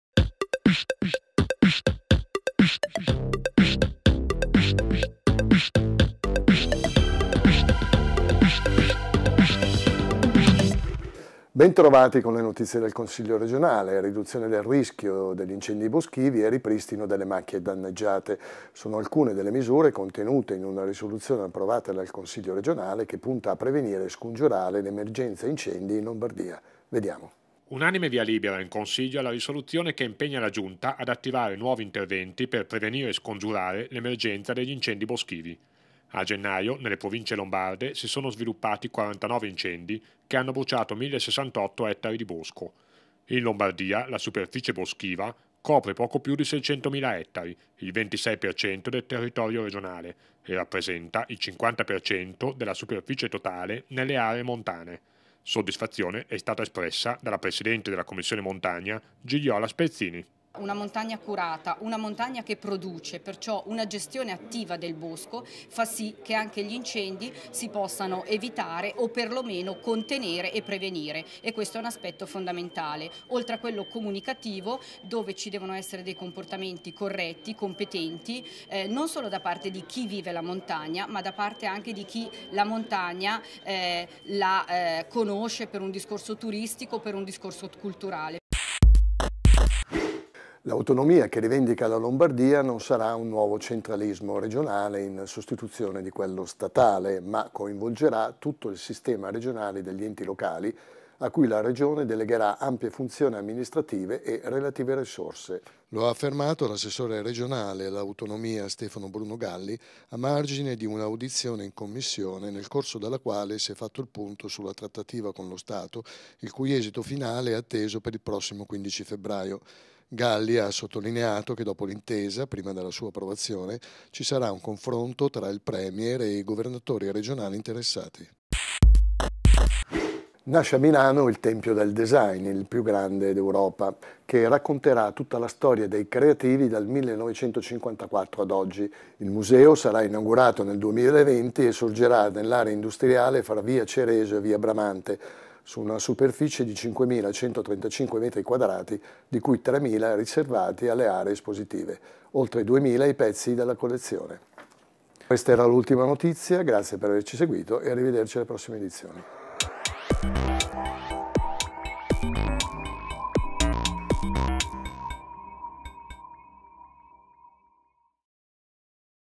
TG WEB 166
TG WEB 166 (4-8 febbraio). In questa edizione: più prevenzione per scongiurare gli incendi. Autonomia: il punto in Commissione. Design, a Milano il museo dei creativi